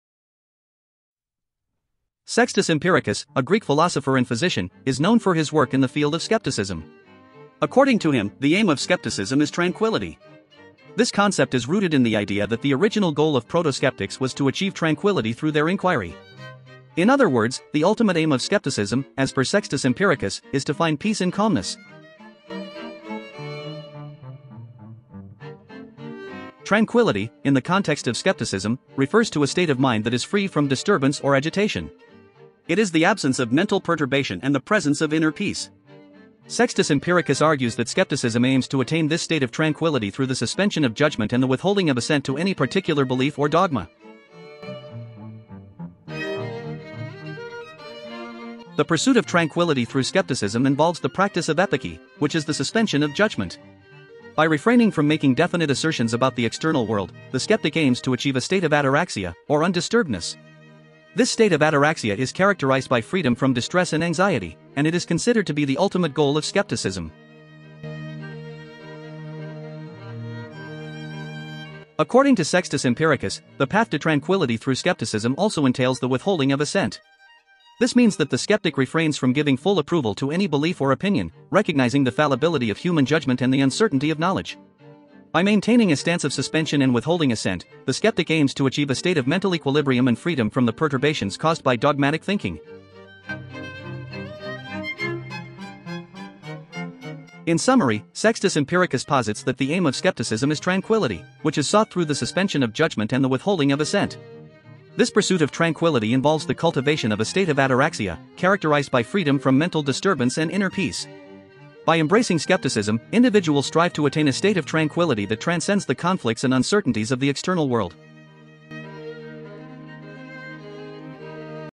What is the aim of skepticism according to Sextus Empiricus __________________? | Philosophy
Sextus Empiricus suggests that the aim of skepticism is tranquility, as it was the original goal for the sake of which the proto-Skeptics began their inquiry. This explanation is the only one that can be found in Sextus, indicating that skepticism is ultimately seeking tranquility.

philosophy fd0d